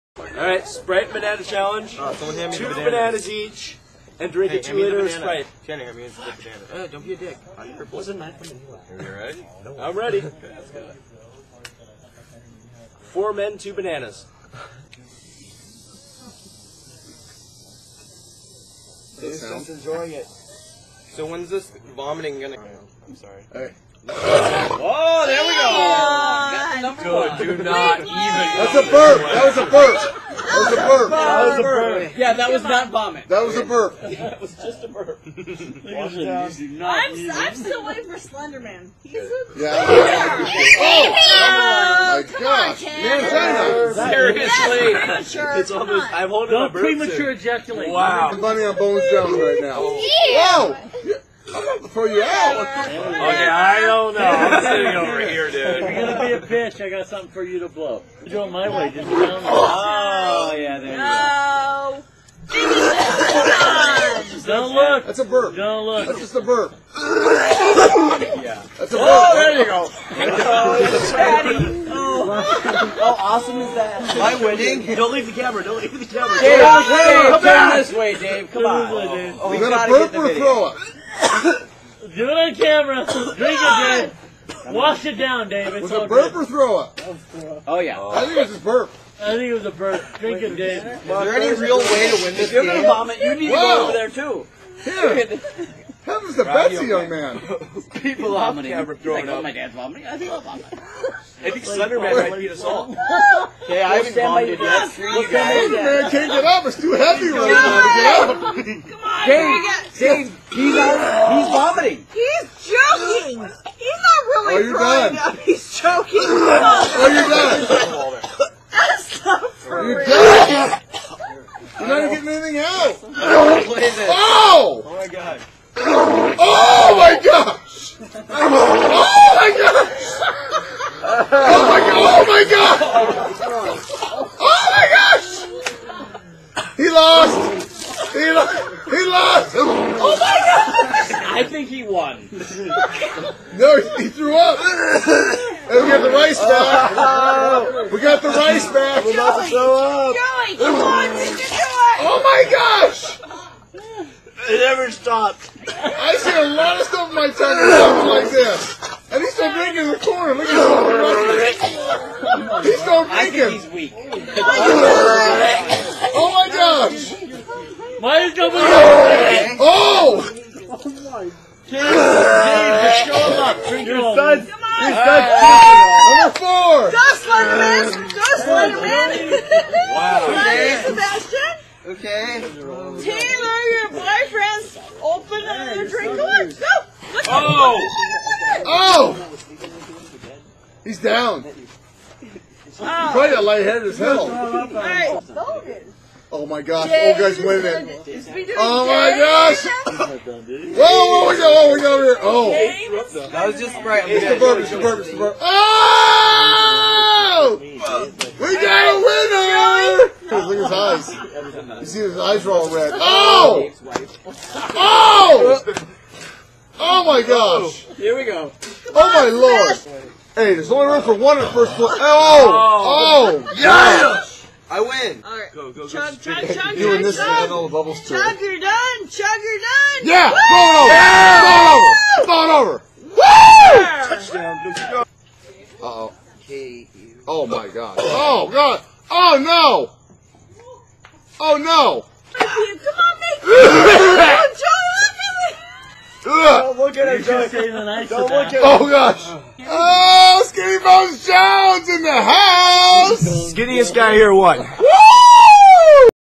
Tosh.0 Banana and Sprite Challenge Fail and Win
Tosh.0 Banana and sprite challenge completed by one of us and failed by the rest.